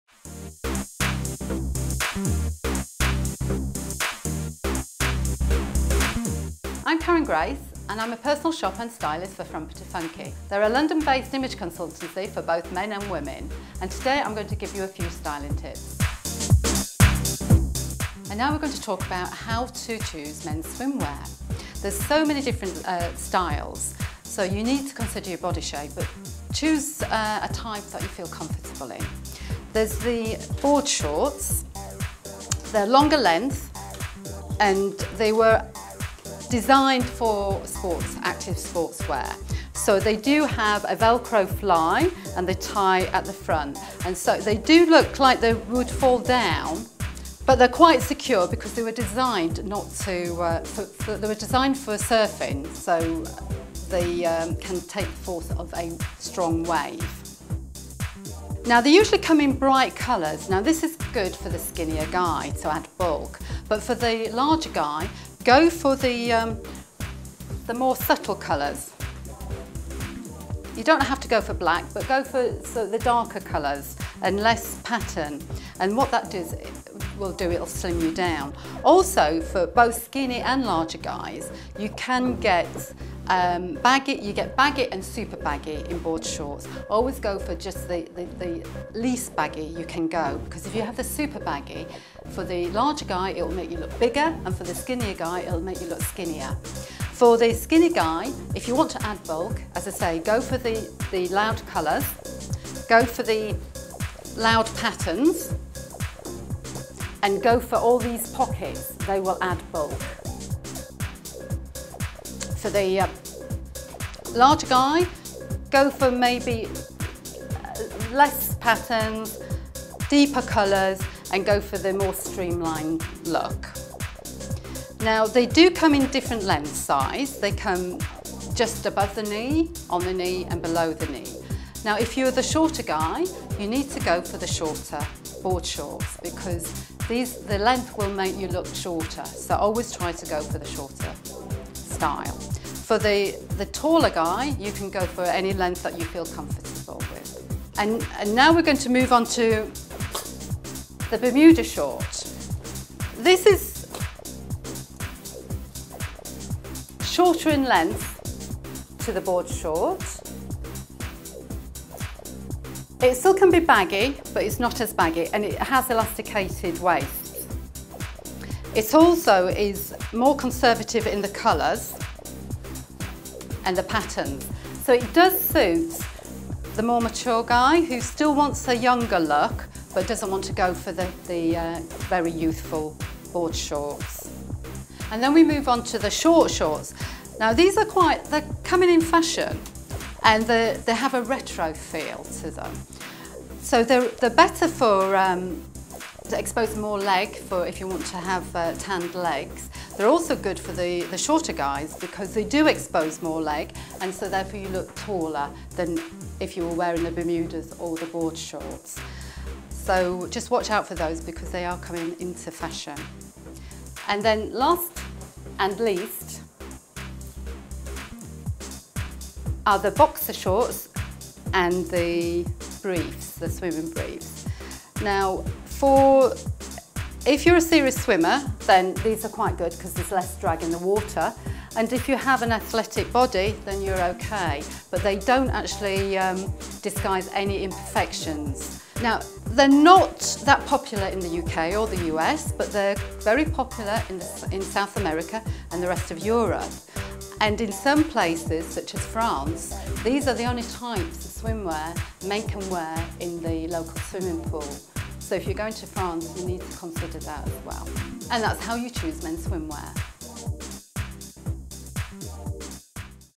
How To Choose Mens Swimwear According To Body Shape
This guide shows you How To Choose Mens Swimwear According To Body Shape.